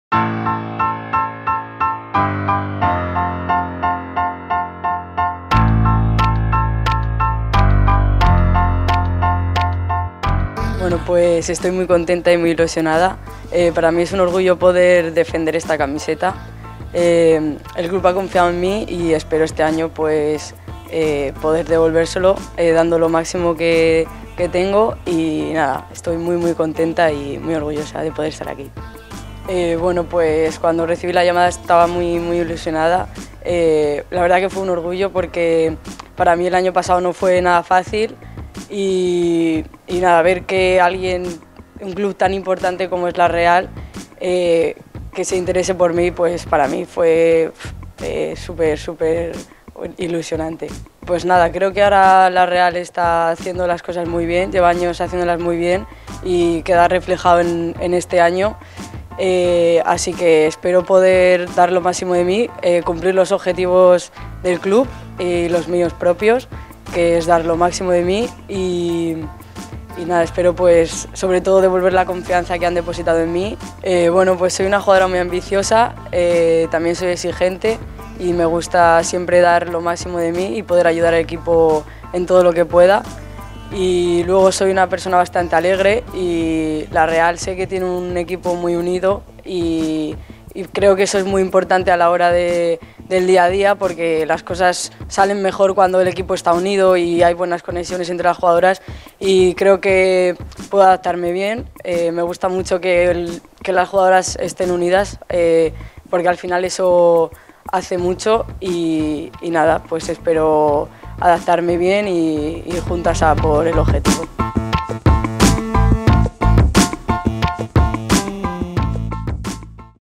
FICHAJE | Alejandra Bernabé: "Orgullosa de vestir estos colores" | Real Sociedad
La Real Sociedad, el Atlético de Madrid y Alejandra Bernabé han llegado a un acuerdo para la cesión de la lateral izquierdo hasta el final de la temporada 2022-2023.

Alejandra Bernabé, de 20 años, es natural de Madrid. El último año estuvo cedida en la SD Eibar. Bernabé llegó al Atlético de Madrid en la temporada 2019 y debutó con el primer equipo en la campaña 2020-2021, desde ese momento la lateral comenzó a formar parte de la primera plantilla.

La jugadora, tras pasar reconocimiento médico en Policlínica Gipuzkoa, y visitar el Reale Arena acompañada por el consejero de la entidad, Alex Uranga se ha mostrado “orgullosa de vestir estos colores”. Bernabé ha destacado que está muy ilusionada. “Ver como un club tan importante como la Real se ha interesado por mí es muy ilusionante y espero devolverle al club toda la confianza”

Realak akordio bat lortu du Atlético de Madridekin Alejandra Bernabe jokalariak utzita jokatzeko hurrengo denboraldian. Klub txuri-urdina aldi berean akordio batera iritsi da ezker hegaleko defentsarekin.

Alejandra Bernabe, 20 urtekoa, Madrilgoa da. Azken denboraldian SD Eibarren jokatu zuen. Bernabé 2019 denboraldian iritsi zen Atlético de Madridera. Lehen taldearekin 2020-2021 denboraldian debutatu zuen eta momentu horretatik aurrera lehen taldearekin aritu zen.

Gipuzkoako Poliklinikan azterketa medikoa egin eta Reale Arena bisitatu ostean, Alex Uranga kontseilariak lagunduta, “kolore hauek janzteaz harro”. Bernabe oso ilusionatua dagoela nabarmendu du. “Reala bezalako klub batek nigan interesa duela ikusteak ilusio handia egin zidan eta klubari konfiantza oso itzultzea espero dut”, azpimarratu du Bernabek.